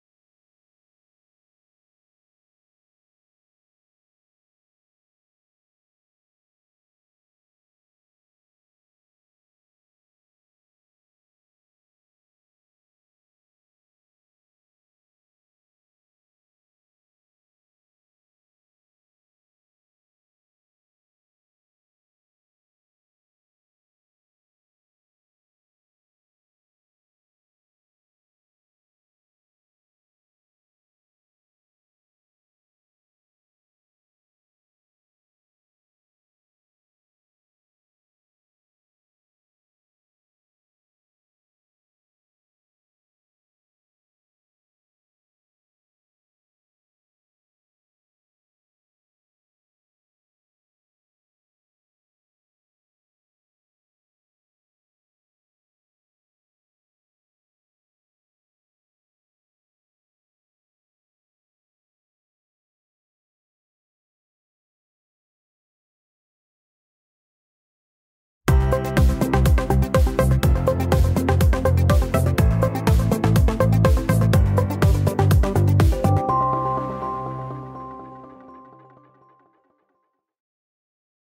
Sieger: Julia Renner mit Chalone - CSI1* Finale Bronze Tour mit Stechen 1,15M...
Gut Sedlbrunn Pöttmes CSI2* - CSI1* 29.09–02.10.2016